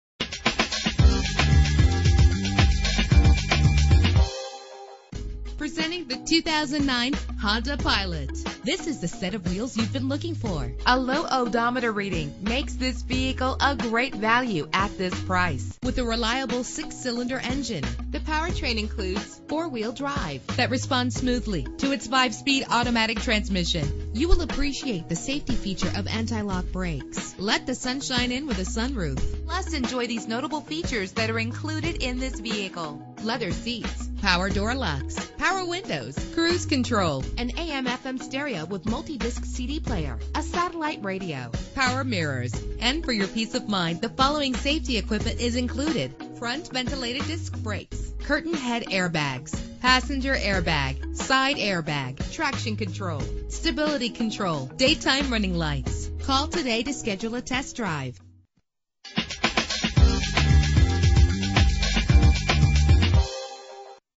2009 Honda Pilot - Vestal NY
Year: 2009
Make: Honda
Model: Pilot
Trim: EX-L
Engine: 3.5 liter 6 cylinder 24 valve
Transmission: 5-Speed Automatic
Color: Gray
Mileage: 0
Address:
3721 Old Vestal Rd